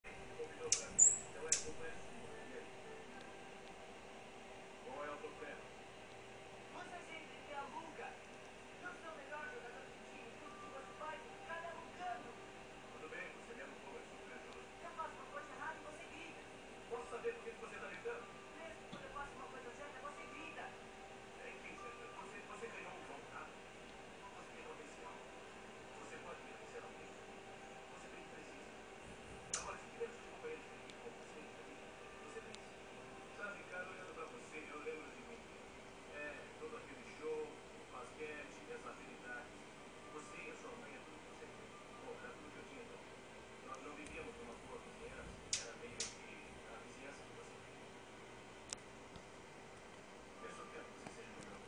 Beija-flor Dorotéia e Jr.
Uma fêmea de beija-flor (Dorotéia) fez ninho na janela do meu quarto , permitiu que eu pegasse o filhote. ( Junior) e ainda pousou na minha mão pra dar comida. Presente de Deus!